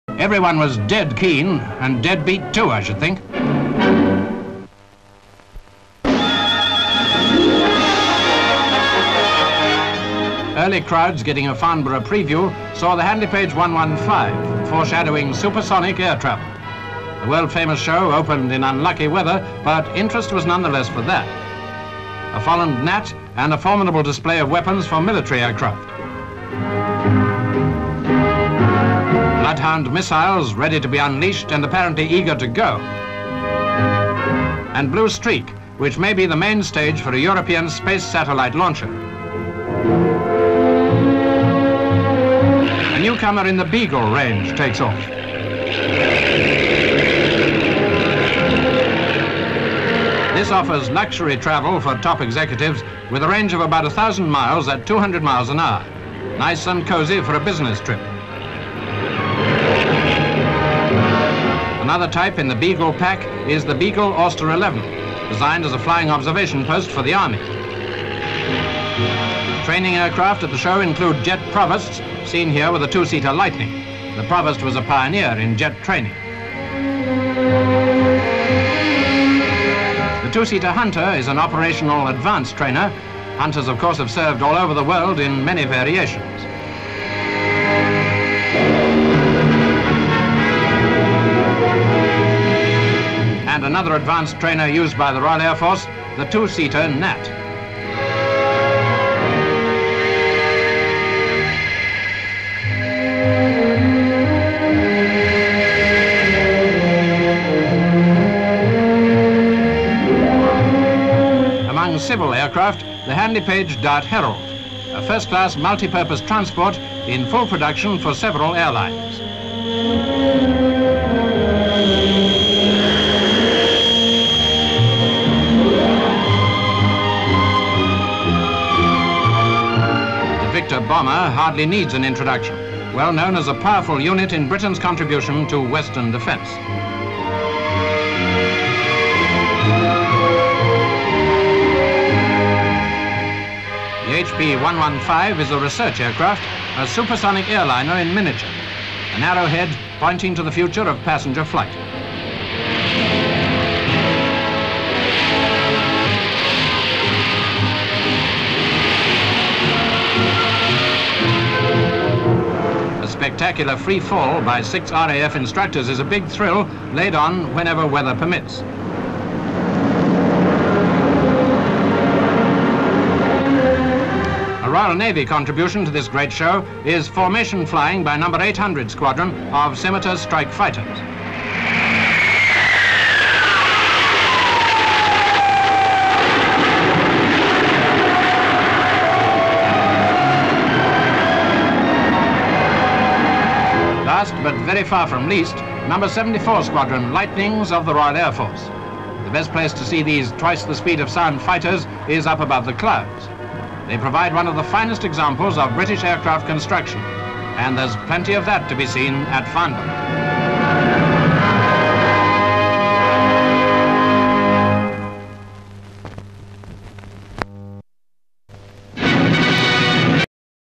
FLIGHT TO THE FUTURE (COLOUR)
(7 Sep 1961) The world-famous Farnborough Show opened in unlucky weather, but interest was none the less for that! A newcomer in the Beagle range offers luxury travel for top executives, another type in the Beagle pack was the Beagle-Auster Eleven. Training aircraft at the show included Jet Provosts, and the two-seater "Hunter", an operational advanced trainer. Another advanced trainer, used by the Royal Air Force, was the two-seater "Gnat". Among civil aircraft: the Handley Page "Dart Herald". The "Victor" Bomber hardly needs an introduction! A spectacular free fall by six RAF instructors was a big thrill, and a Royal Navy contribution to this great show was formation flying by Number 800 Squadron of "Scimitar" strike-fighters. All these provide one of the finest examples of British Aircraft construction, and there's plenty of that to be seen at Farnborough.